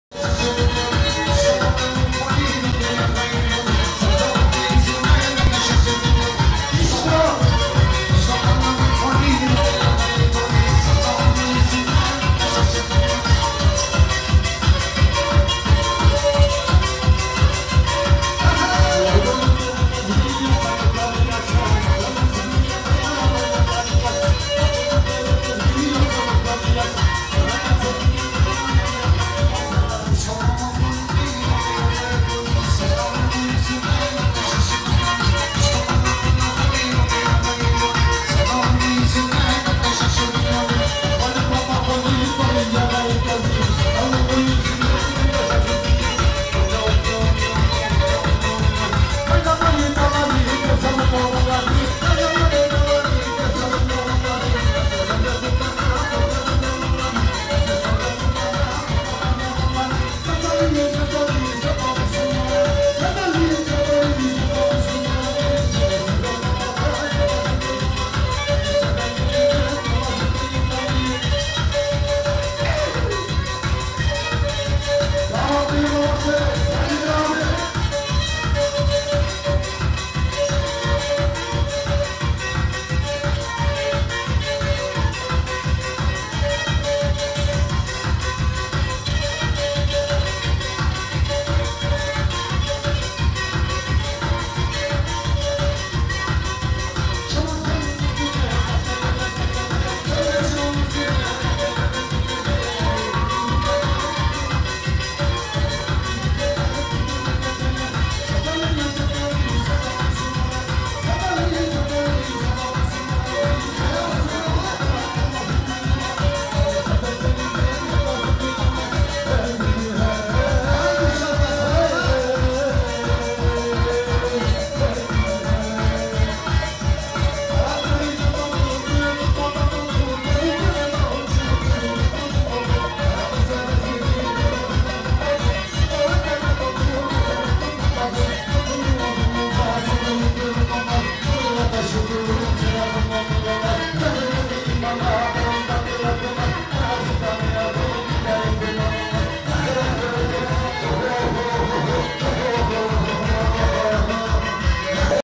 MİRAÇ ÇAKMAKÇİ 0536.5836754 TRABZON KASTAMONU AŞKIM SALON SELİM YASEMİN ÇOBAN KUŞ
YER İST HARAÇÇI AŞKIM DÜĞÜN SALONU. TRABZON. KASTAMONU DÜĞÜNÜ. DAMATIN BABASI ALİRİZA ÇOBAN ABİMİZİN OĞLU SELİM ÇOBAN VE GELİN YASEMİN KUŞ AİLELERİ MUTLULUKLAR DİLERİZ. KEMENCE OKUMADA MİRAÇ ÇAKMAKÇİ KEMENCECİ İRT.